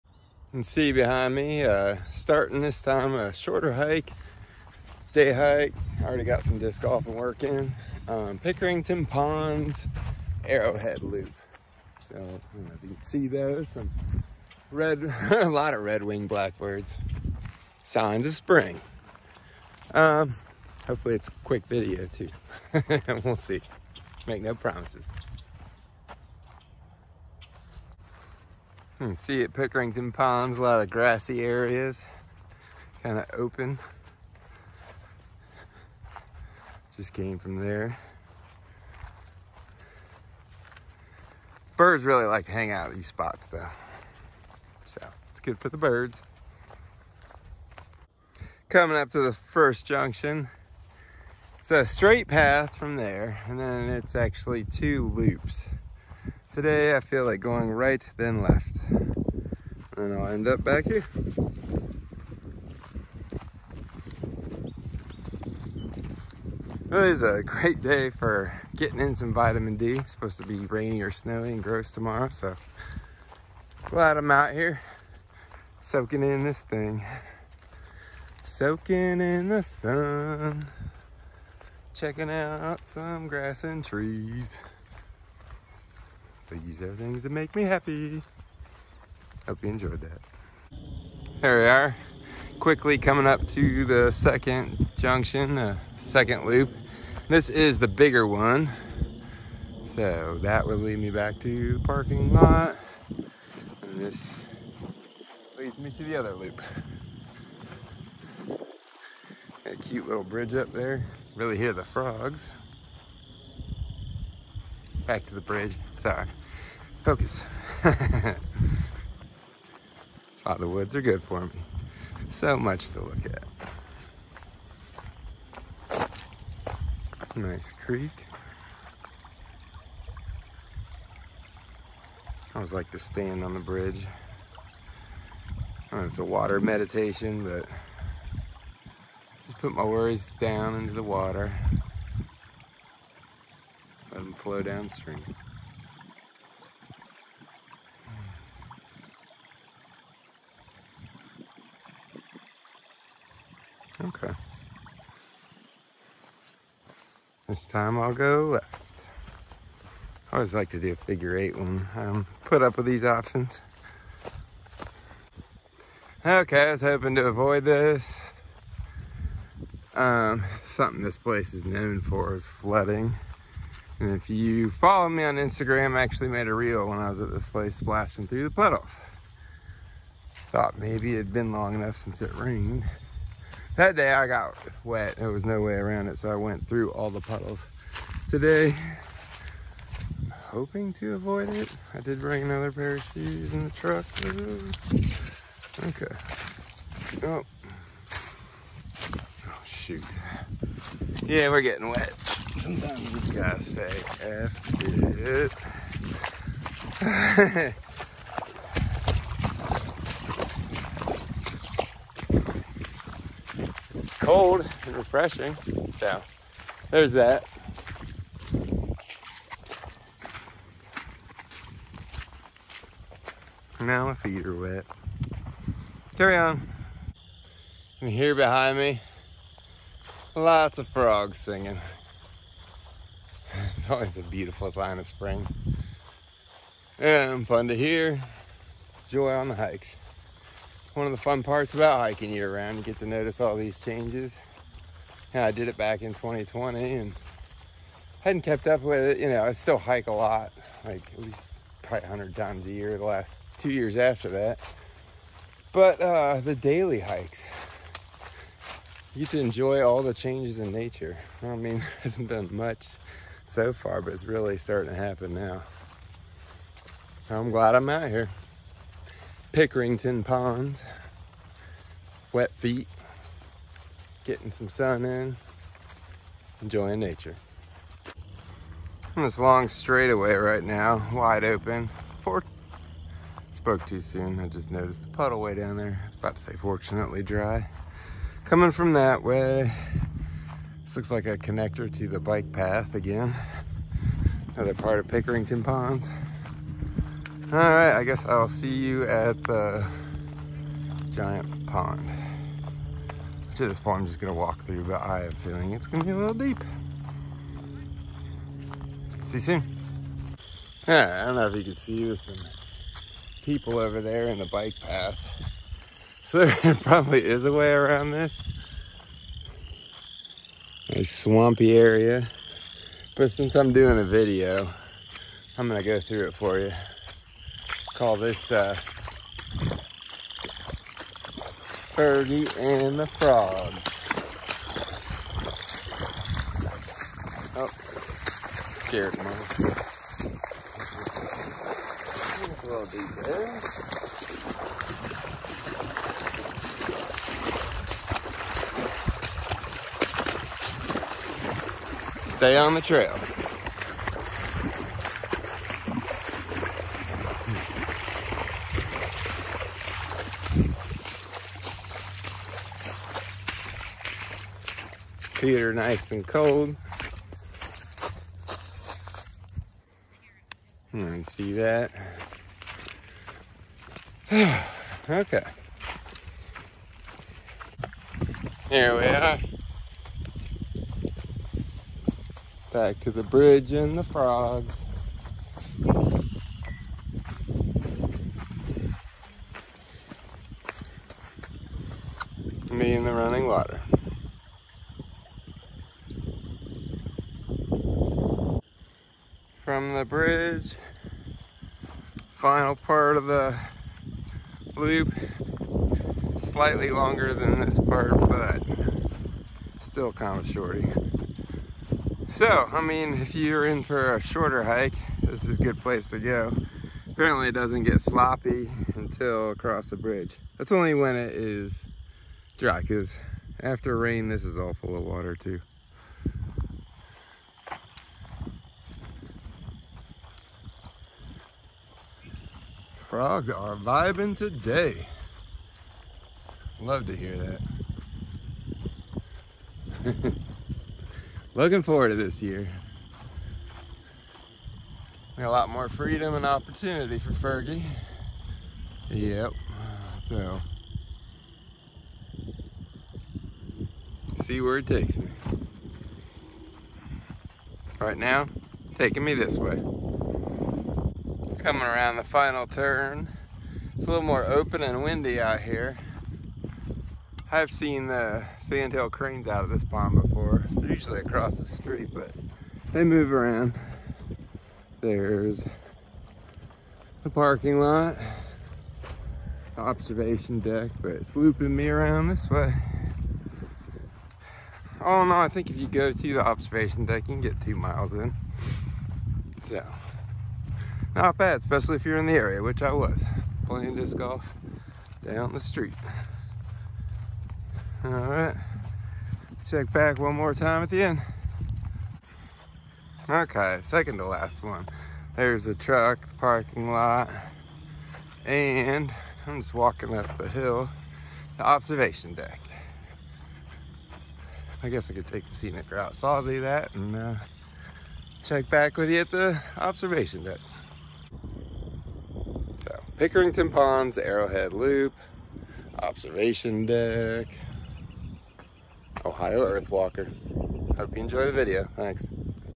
Fergie and the Frogs :) Pickerington Ponds Metropark, March 9 / Day 68 of hiking in ‘23
Oftentimes I try to correlate my daily hikes with the area I’m in. I was in Pickerington so I headed to the Arrowhead Loop Trail at Pickerington Ponds. It was a sunny day, but there were still large puddles/ponds on the trails. Oh, and frogs croaking. Lots of them. 🐸